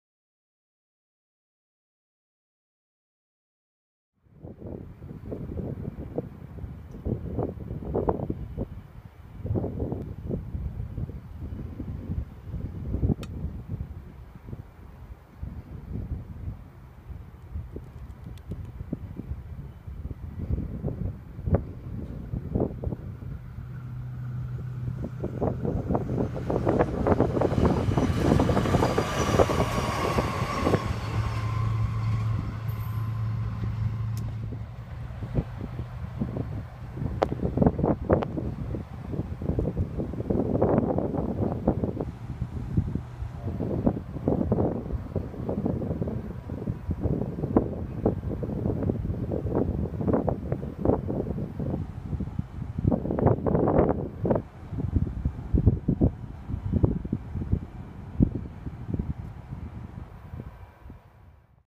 Metro IEV100 (Track Inspection) in Galvin (20/5/18)
In this video, we see Metro IEV100 passing through Galvin during track inspections on Sunday 21st of May 2018.